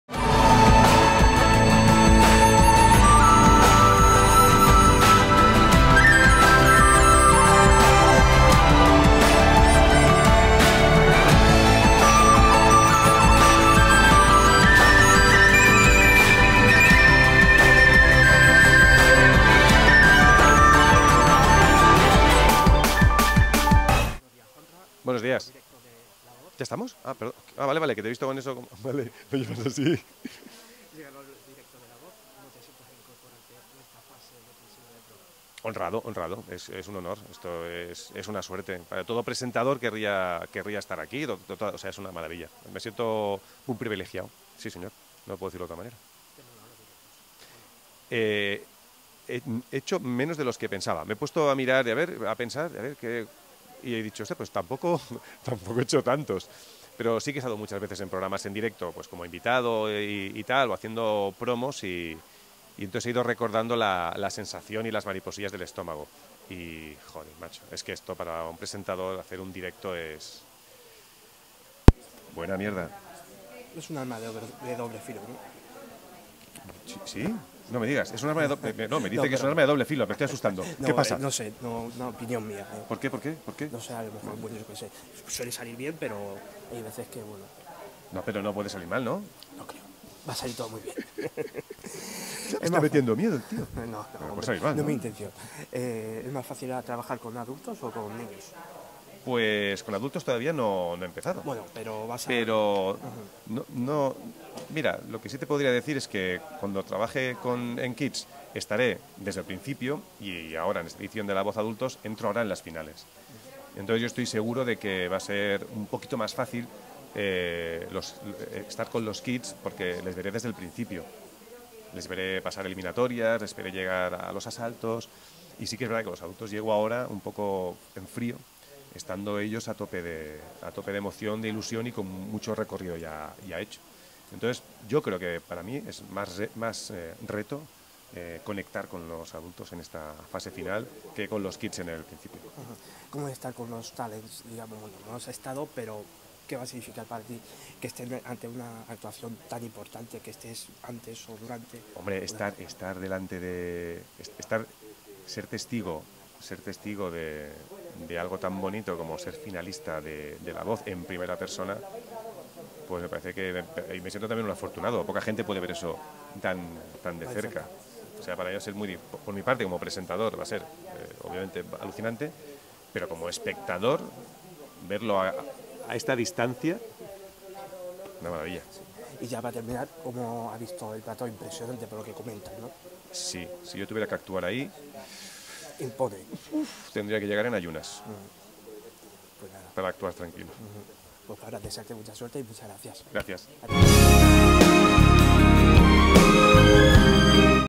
Directos 'La Voz' (Antena 3) | Entrevista Juanra Bonet | Destino Talento
'La Voz' entra en su fase decisiva con los directos.